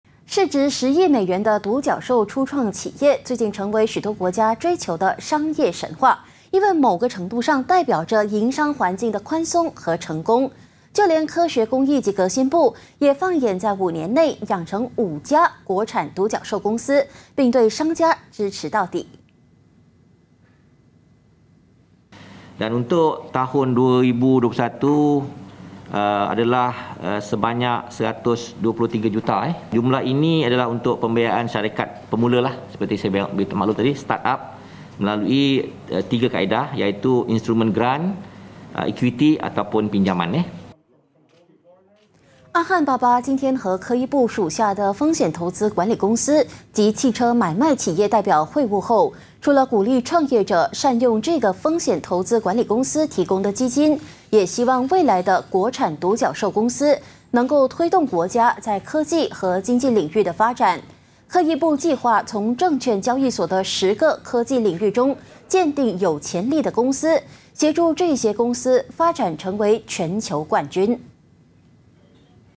【5年养成5家独角兽公司 科艺部物色潜力企业】
市值10亿美元的独角兽初创企业，最近成为许多国家追求的商业神话，因为某个程度上，代表着营商环境的宽松和成功，就连科学、工艺及革新部也放眼在5年内养成5家国产独角兽公司，并对商家支持到底！